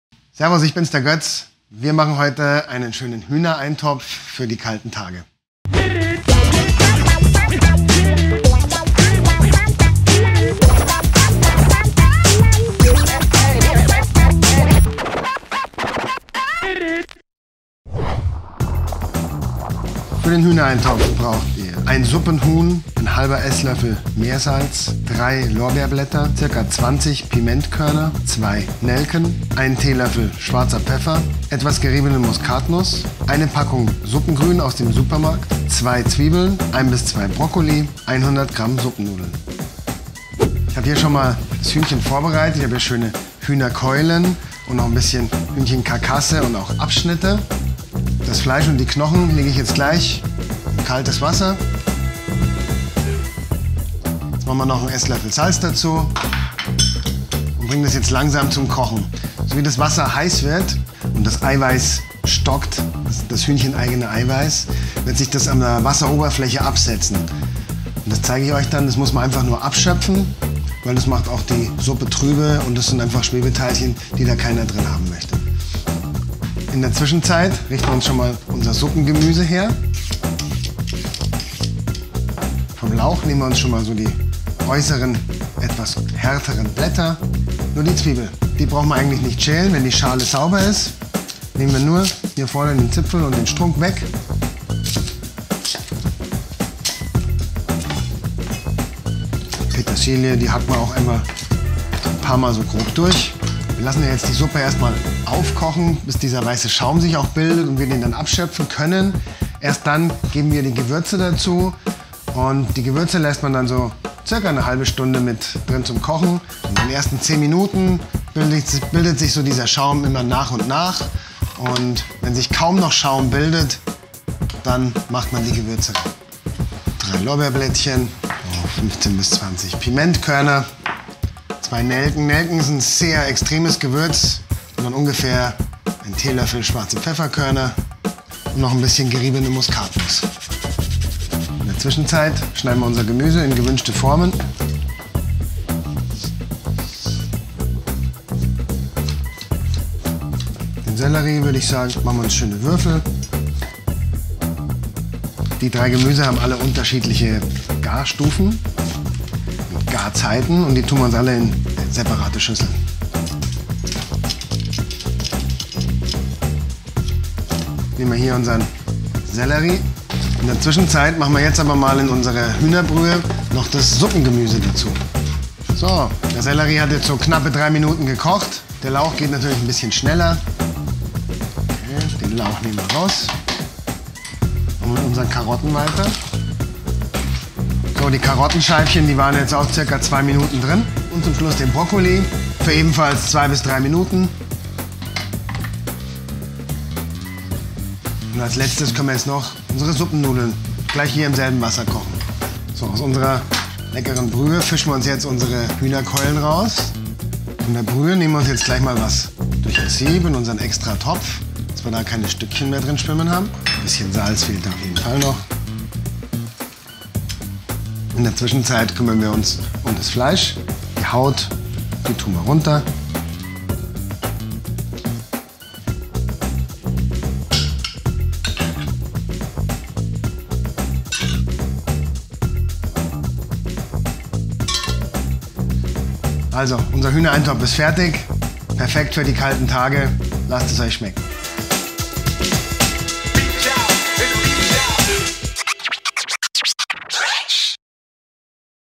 Hühnerbrühe selber machen: Winter Rezept vom Küchenteufel
Hühnerbrühe ist das klassische Gericht bei Erkältungen.
Gerade im Winter wärmt sie uns auf und stärkt unsere Abwehrkräfte. Kochprofi Götz Beller zeigt Dir in diesem Video ein einfaches Rezept seiner Hühnerbrühe.